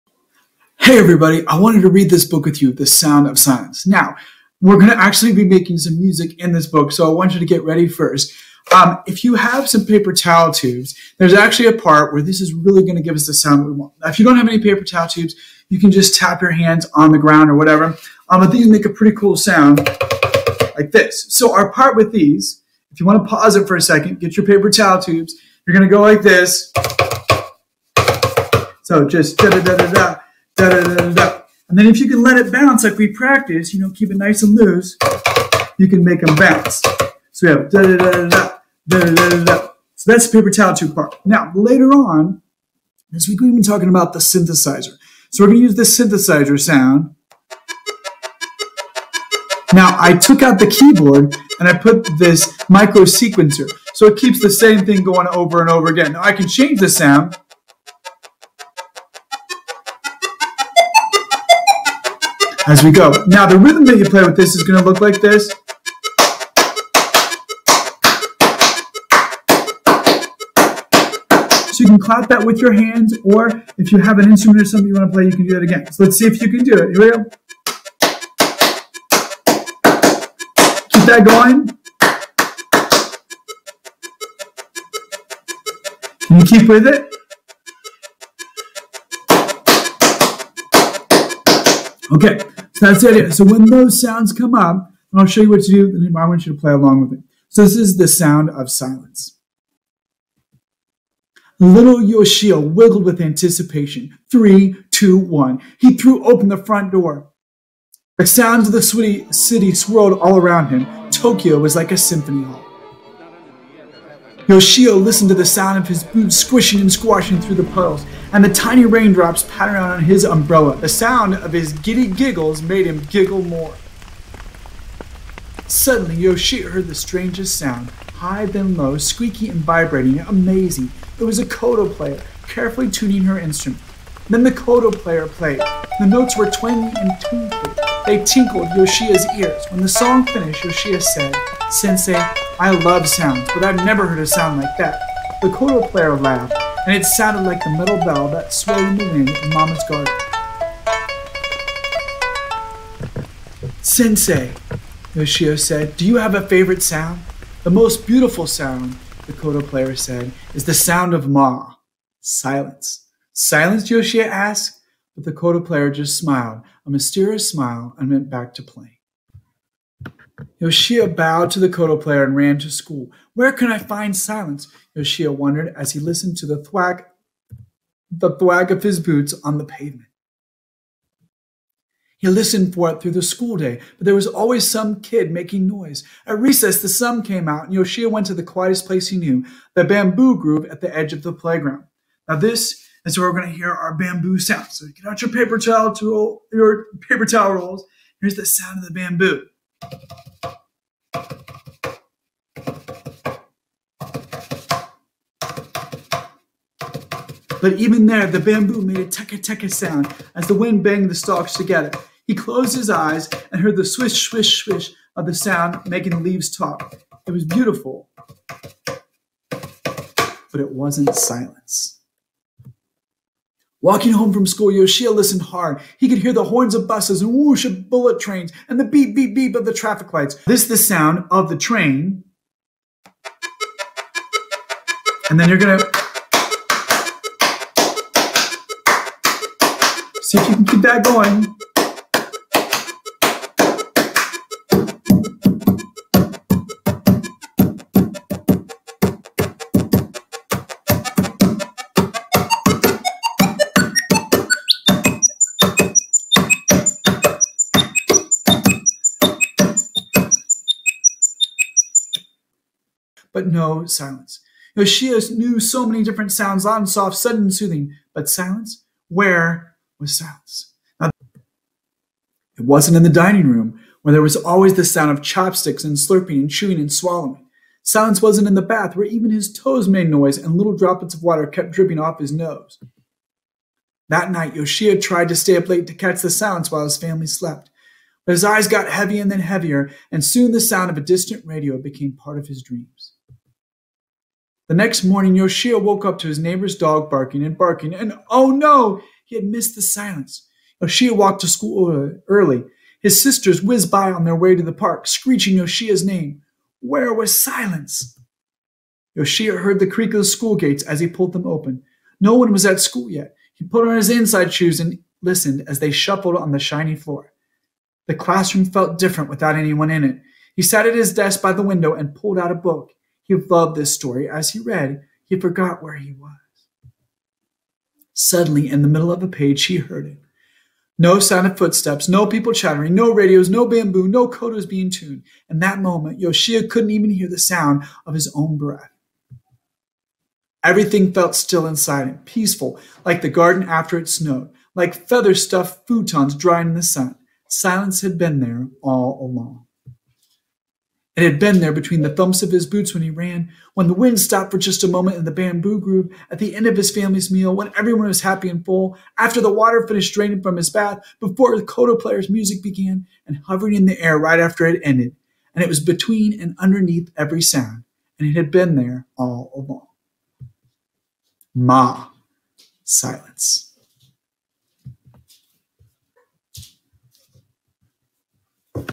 Sound of Silence Book
Read aloud for the book "Sound of Silence." See how the synthesizers are used to make the sound of the city. What sounds can you make to add to the book?